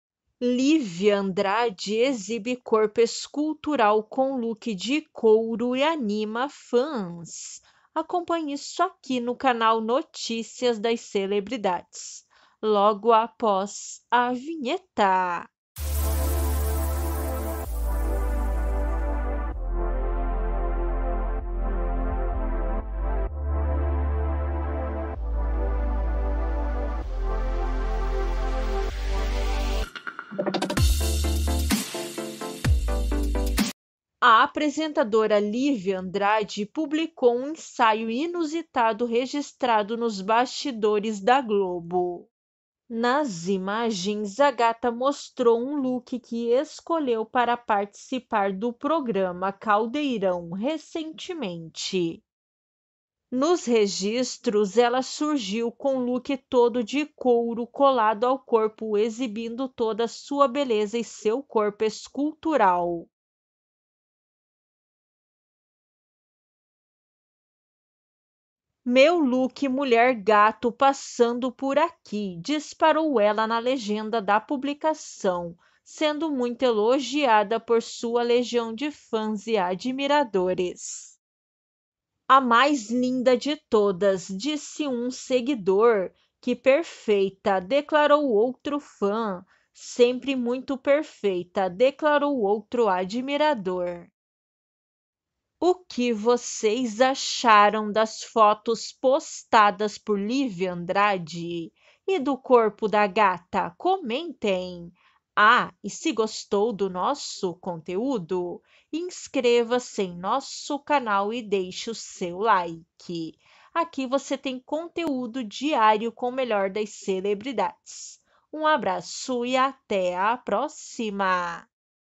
LÍVIA ANDRADE EXIBE CORPO ESCULTURAL COM LOOK DE COURO E ANIMA FÃS!
A apresentadora Lívia Andrade publicou um ensaio inusitado registrado nos bastidores da Globo.

Nas imagens, a gata mostrou um look que escolheu para participar do programa Caldeirão recentemente.

Nos registros, ela surgiu com um look todo de couro colado ao corpo exibindo toda a sua beleza e seu corpo escultural.

"Meu look Mulher Gato passando por aqui", disparou ela na legenda da publicação, sendo muito elogiada por sua legião de fãs e admiradores.

"A mais linda de todas", disse um seguidor; "Que perfeita", declarou outro fã; "Sempre muito perfeita", declarou outro admirador.

Assistam o vídeo até o final e comentem o que vocês acharam das fotos postadas por Lívia Andrade! E do corpo da gata? Comentem!